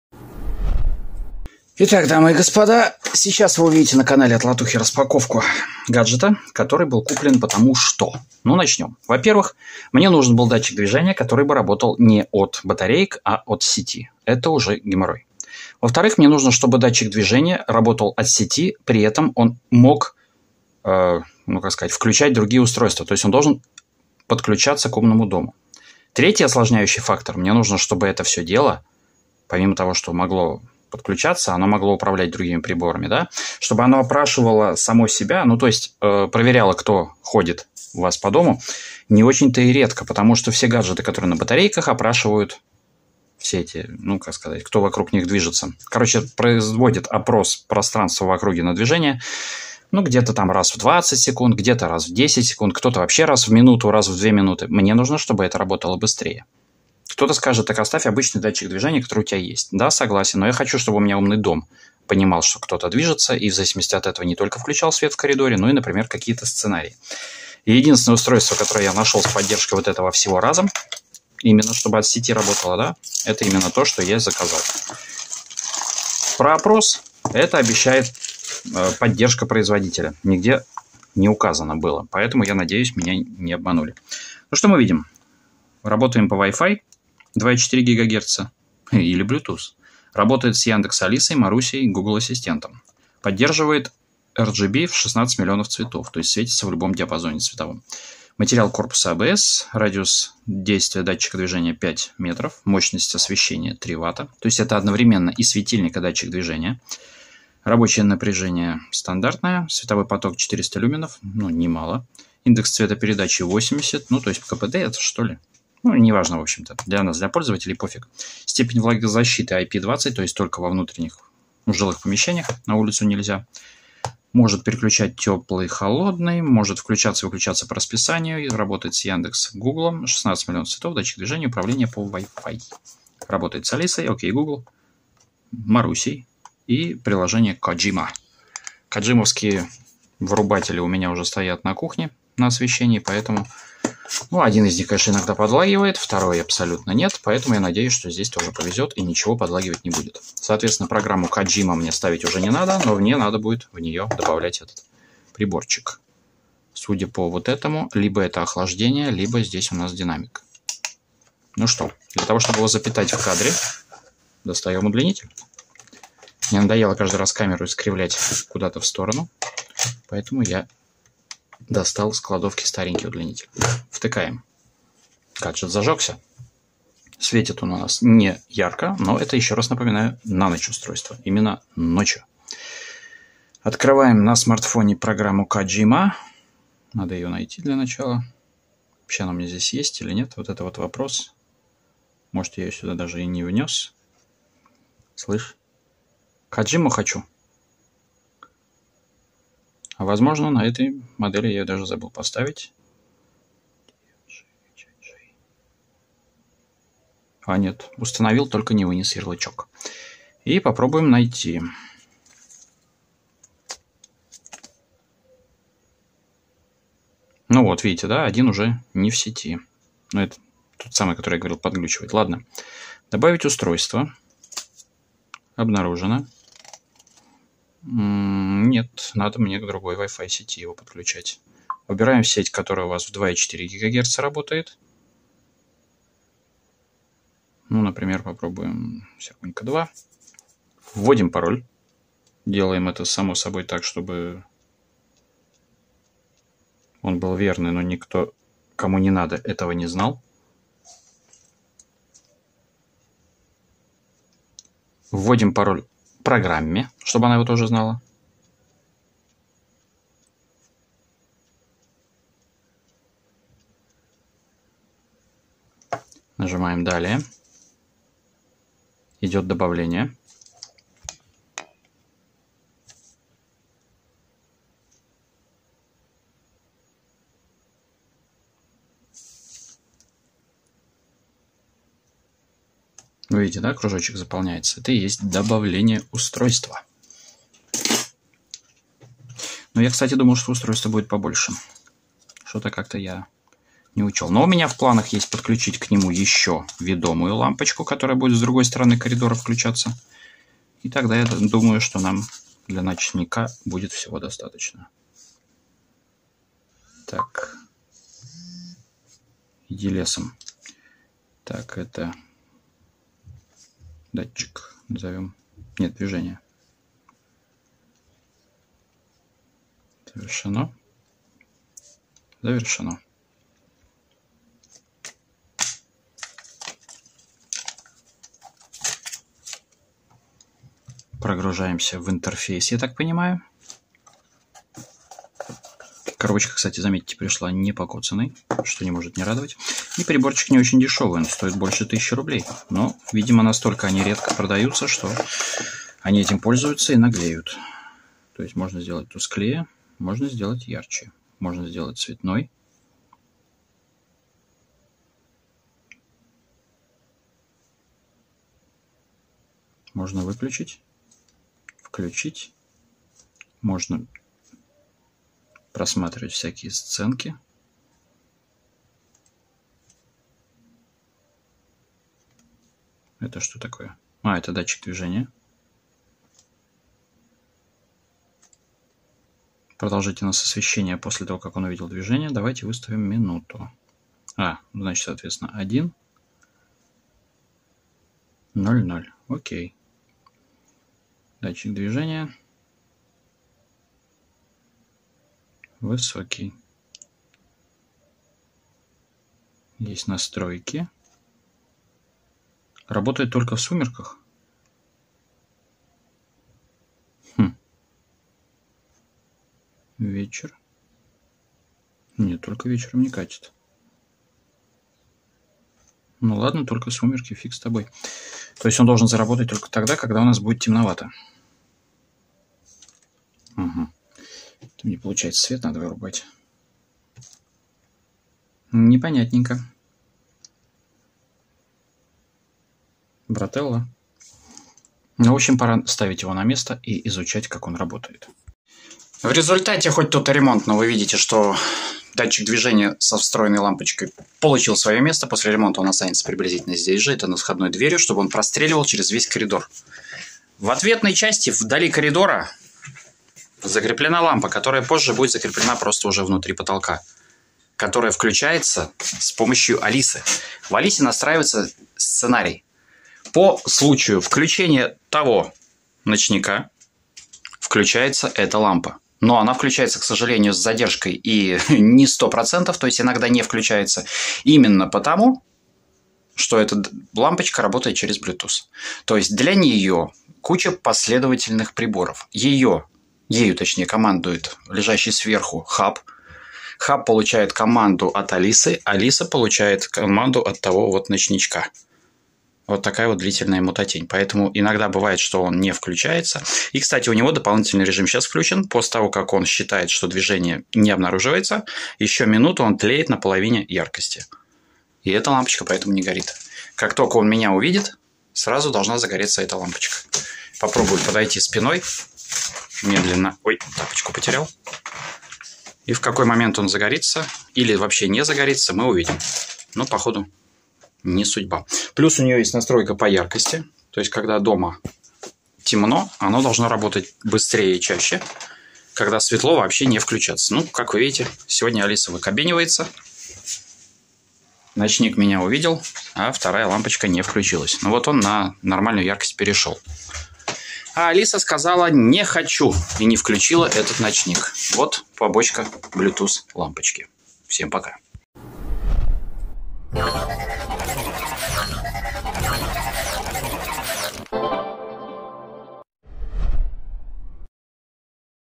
Умный светодиодный ночник с датчиком движения RGB с Wi-Fi, Яндекс Алисой, Марусей, 220V 3W
Снято и сведено на Blackview BL6000 Pro

- Управление по Wi-Fi 2.4ГГц, Bluetooth
- Работает с Яндекс Алисой, Марусей, Google Assistant
- 16 млн. цветов в режиме RGB
- Цветовая температура от 2700K до 5700K
- Материал корпуса – ABS пластик
- Дальность действия датчика движения 5М
- Мощность - 3Вт
- Раб. напряжение 110-240В 50/60 Гц
- Световой поток - 400 лм
- Индекс цветопередачи - 80
- Угол обзора 120 градусов
- Срок службы 25000 часов
- Степень влагозащиты IP20
- Д*Ш*В - 55*70*75 мм, Вес - 65 г.